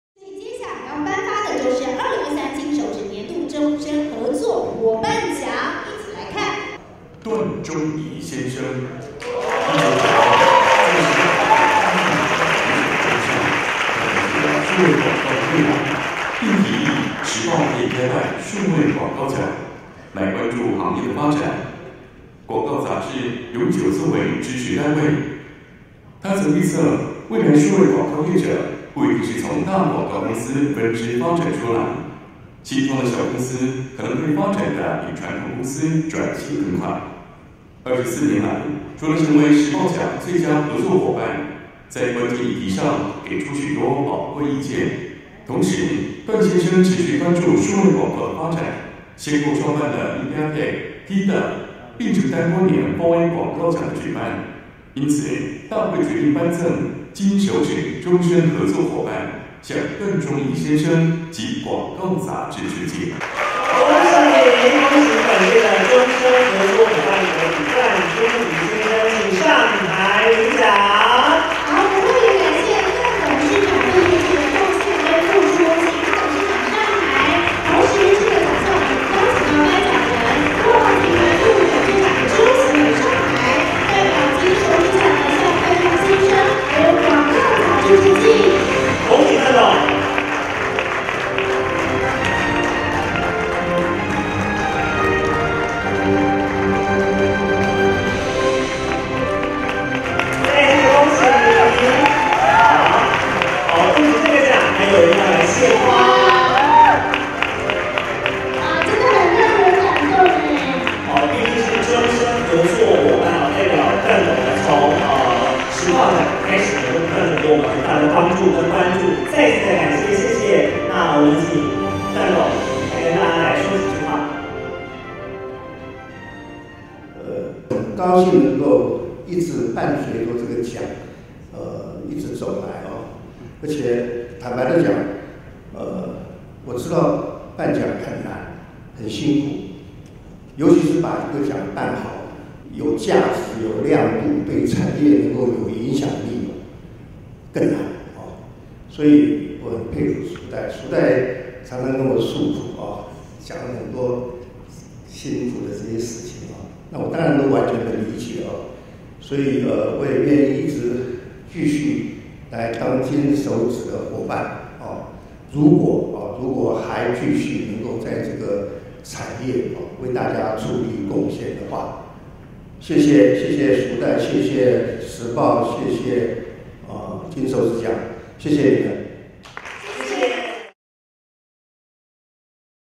滾石文化董事長 段鍾沂《2023年度金手指終身合作夥伴奬》 得獎感言
恭喜段董事長獲得終身合作夥伴獎！

🏆【2023年度金手指終身合作夥伴奬】
頒獎人：旺旺集團副董事長 周錫瑋
得獎人：滾石文化 董事長 段鍾沂